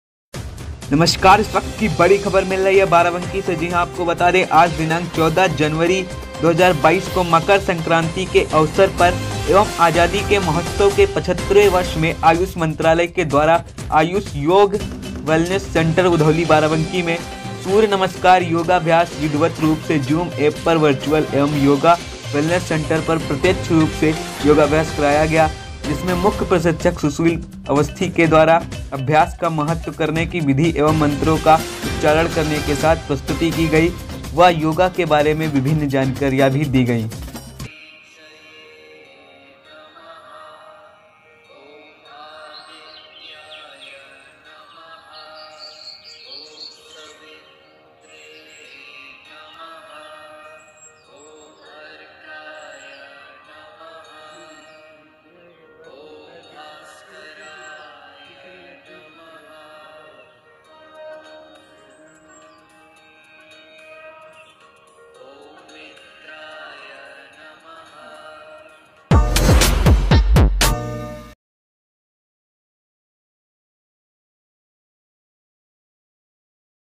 मकर संक्रांति व आजादी के अमृत महोत्‍सव पर बाराबंकी में सूर्य नमस्‍कार आयोजित
सूर्य नमस्‍कार जूम एप, ऑनलाईन किया गया